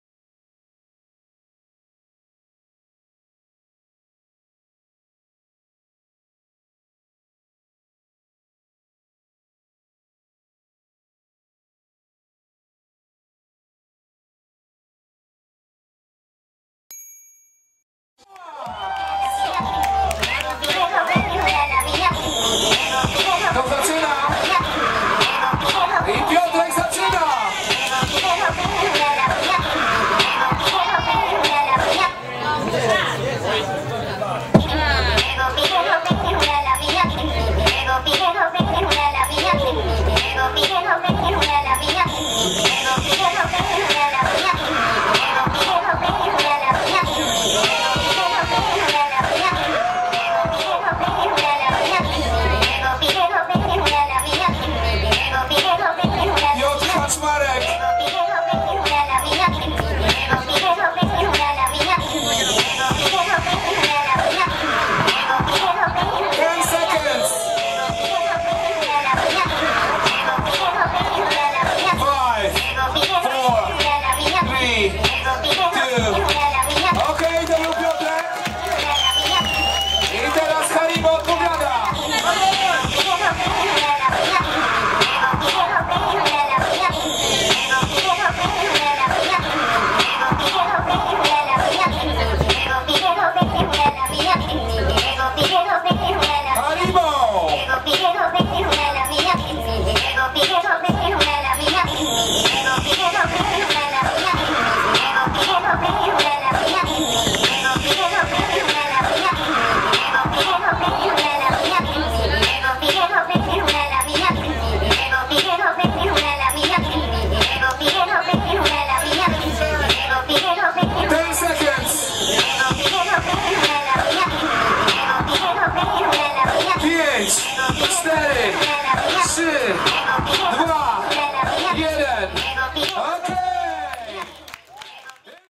Top16 Hip-Hop 1vs1 - Haribo vs Piotrek | Gdańsk Dźwiga Muze 2013
Top16 Hip-Hop 1vs1 Gdańsk Dźwiga Muze 2013

Haribo vs Piotrek

Winner: Piotrek